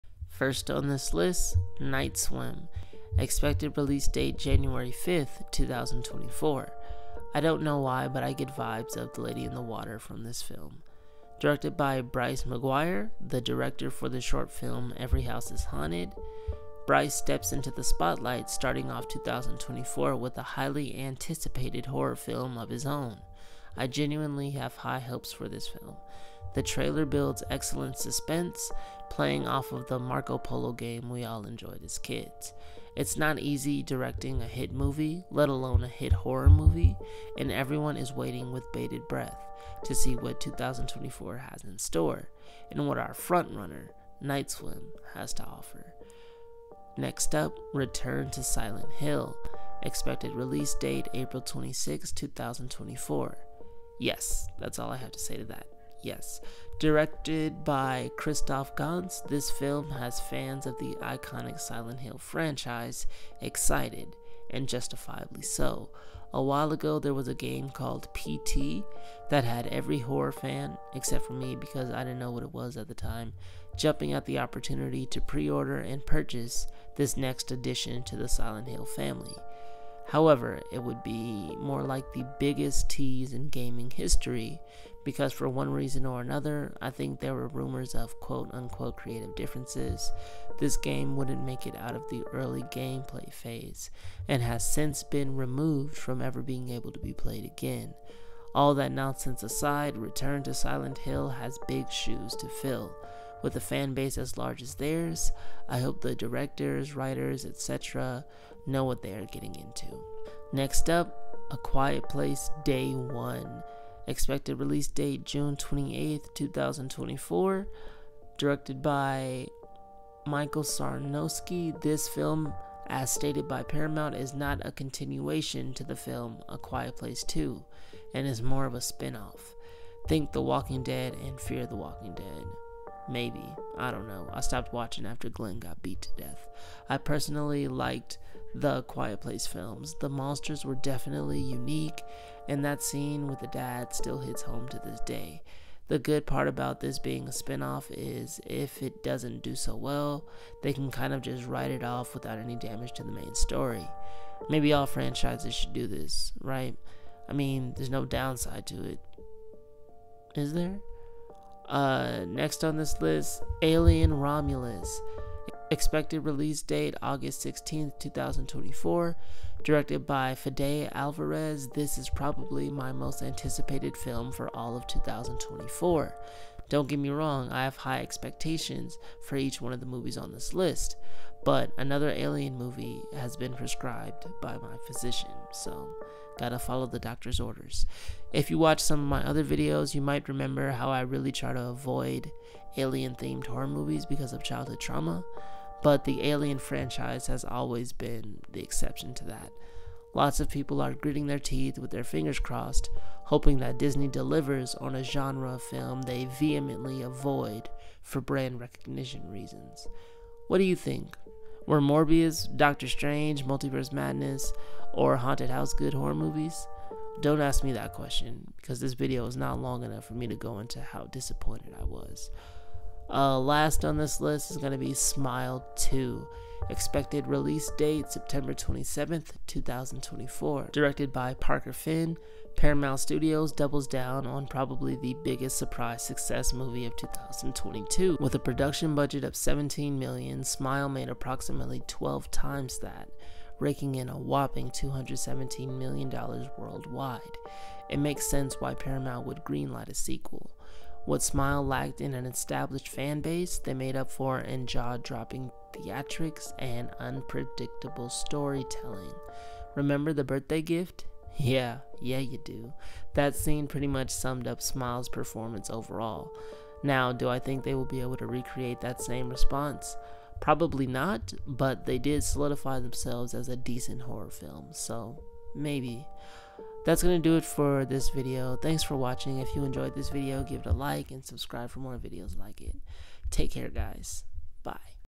5 of My Most Anticipated Horror Films for 2024 horrorfilms #2024
Just a note, the trailers are from previous movies since there arent any for the ones i put in this video so yeah. just wanted to be as transparent as possible.